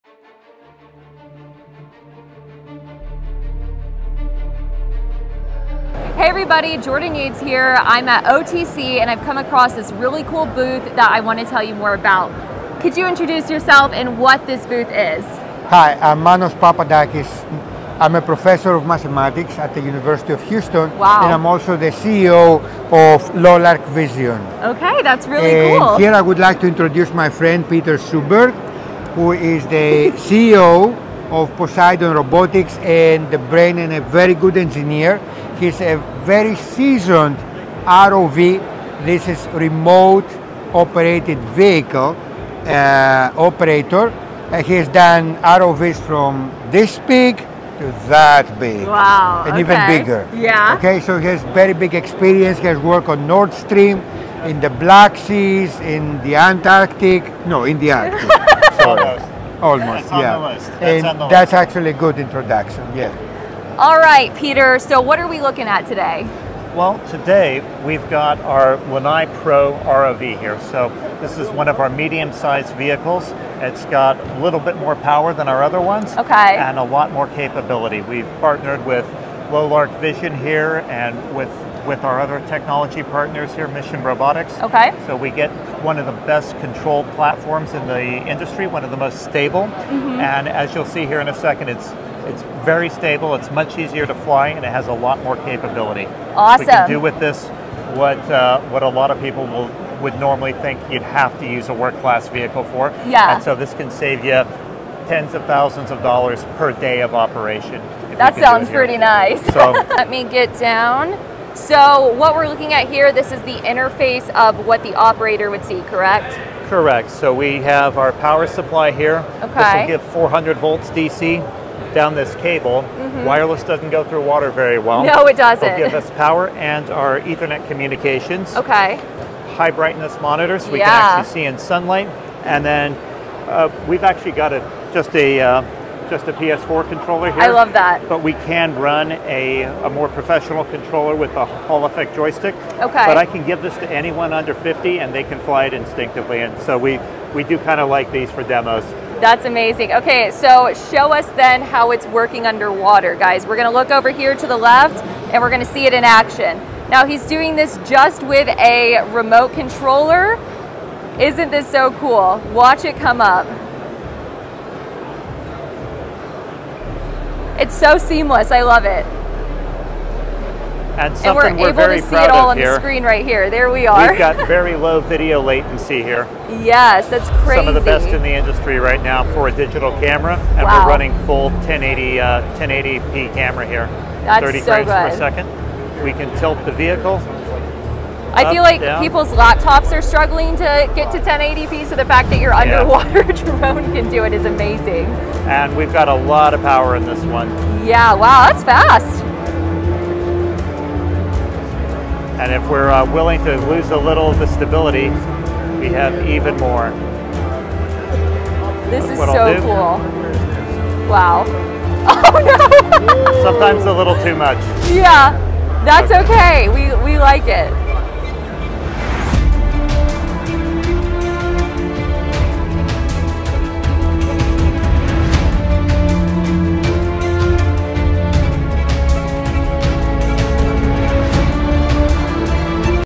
Underwater Remote Operated Vehicle (ROV)
Poseidon Robotics - ROV Demo at Offshore Technology Conference 2023

I went to the 2023 Offshore Technology Conference in Houston, TX and came across the Poseidon Robotics underwater ROV. This booth was awesome! I got to drive the ROV with just a playstation controller.